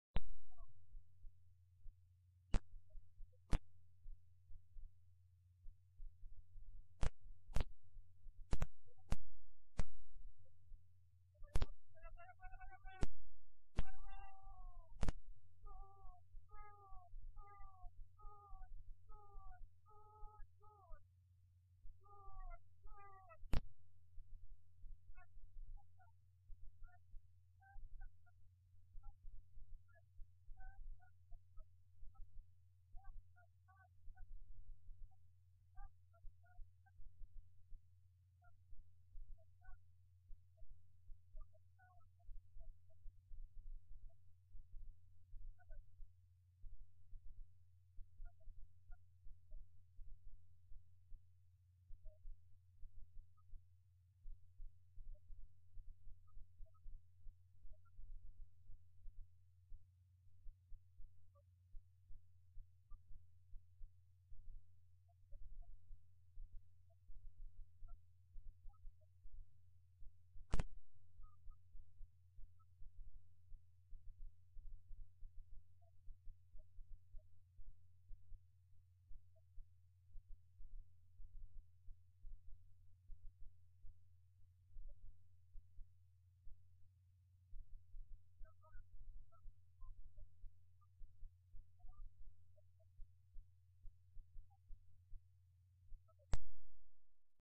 Jordan Vs Uzbekistan-- - 2:0 First goal
Jordan Vs Uzbekistan- 16/08/12  friendly football match